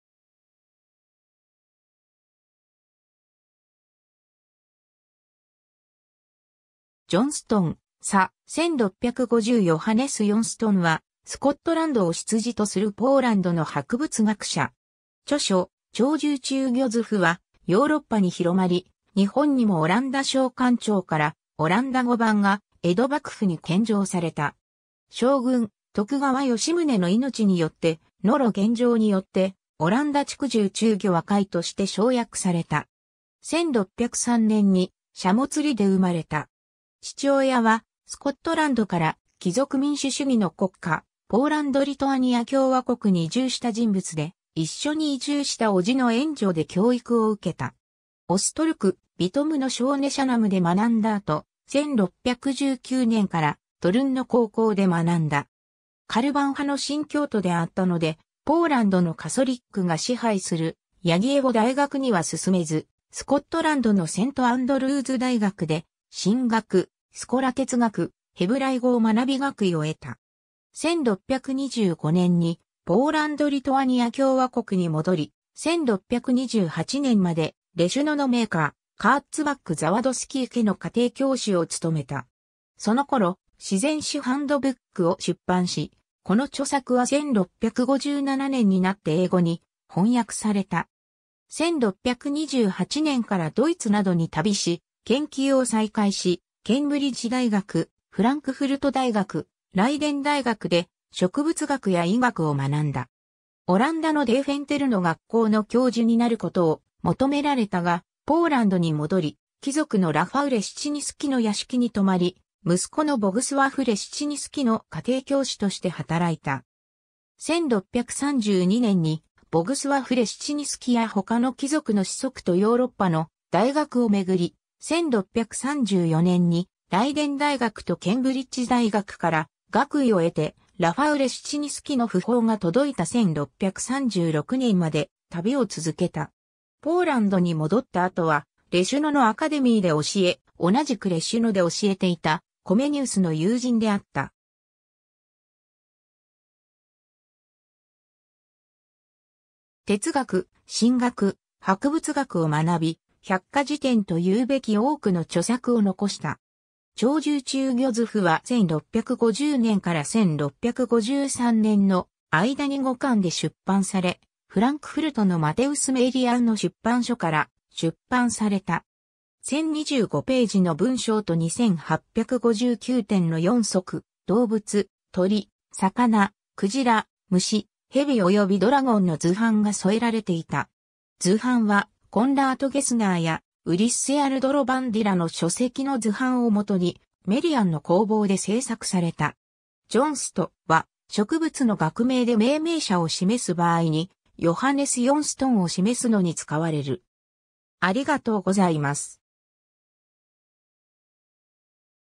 ヨハネス・ヨンストン
セント・アンドルーズ大学出身の人物
Jonston, ca. 1650 ヨハネス・ヨンストン（Johannes Jonston、ポーランド名:　Jan Jonston、イギリス名:John Jonston、ラテン語名: Johannes Jonstonus、1603年9月15日 – 1675年）は、スコットランドを出自とするポーランドの博物学者。
著書、『鳥獣虫魚図譜』（"Historiae naturalis de quadrupedibus libri, cum aeneis figuris"など5巻にわたる図説）はヨーロッパに広まり、日本にもオランダ商館長からオランダ語版が江戸幕府に献上された。
将軍、徳川吉宗の命によって、野呂元丈によって、『阿蘭陀畜獣虫魚和解』（1741年）として抄訳された。
 1603年にシャモツリで生まれた。
父親はスコットランドから貴族民主主義の国家、ポーランド・リトアニア共和国に移住した人物で、一緒に移住した叔父の援助で教育を受けた。
オストルク（Ostroróg）、ビトムのSchoenaichianumで学んだ後、1619年からトルンの高校で学んだ。
カルヴァン派の新教徒であったのでポーランドのカソリックが支配するヤギェウォ大学には進めず、スコットランドのセント・アンドルーズ大学で、神学、スコラ哲学、ヘブライ語を学び学位を得た。
 1625年にポーランド・リトアニア共和国に戻り、1628年までレシュノの名家、Kurtzbach-Zawadski家の家庭教師を務めた。
その頃『自然史ハンドブック』（"Enchiridion historiae naturalis"）を出版し、この著作は1657年になって英語に翻訳された。
 1628年からドイツなどに旅し、研究を再開し、ケンブリッジ大学、フランクフルト大学、ライデン大学で植物学や医学を学んだ。
オランダのデーフェンテルの学校の教授になることを求められたが、ポーランドに戻り、貴族のラファウ・レシチニスキの屋敷に留まり、息子のボグスワフ・レシチニスキの家庭教師として働いた。
1632年にボグスワフ・レシチニスキや他の貴族の子息とヨーロッパの大学を巡り、1634年にライデン大学とケンブリッジ大学から、学位（M.D.やPh.D.）を得て、ラファウ・レシチニスキの訃報が届いた1636年まで旅を続けた。
ポーランドに戻った後はレシュノのアカデミーで教え、同じくレシュノで教えていたコメニウスの友人であった。
 哲学、神学、博物学を学び、百科事典というべき多くの著作を残した。
 「鳥獣虫魚図譜」は1650年から1653年の間に5巻で出版され、フランクフルトのマテウス・メーリアン（Matthäus Merian）の出版所から出版された。
1025ページの文章と2859点の四足動物、鳥、魚、鯨、虫、蛇およびドラゴンの図版が添えられていた。
図版はコンラート・ゲスナーやウリッセ・アルドロヴァンディらの書籍の図版をもとにメリアンの工房で製作された。
 Jonst.は、植物の学名で命名者を示す場合にヨハネス・ヨンストンを示すのに使われる。